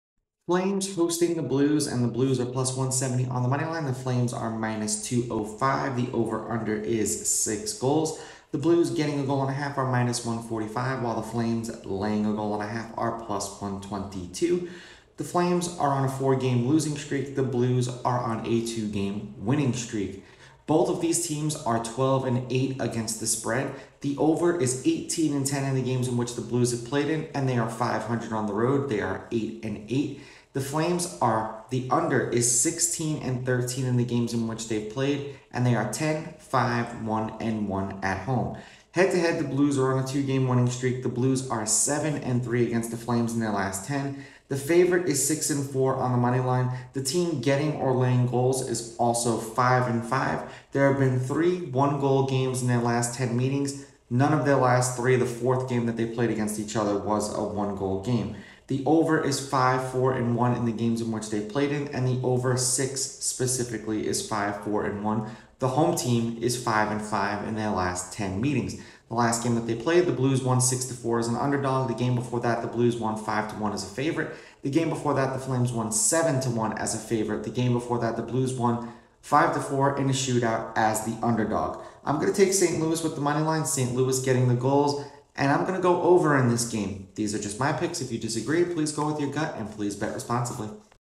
Calgary Flames VS St. Louis Blues 12/16 FREE NHL Sports Betting Info & My Pick/Prediction
FLAMES VS BLUES PREDICTION. FLAMES VS BLUES FREE PICK.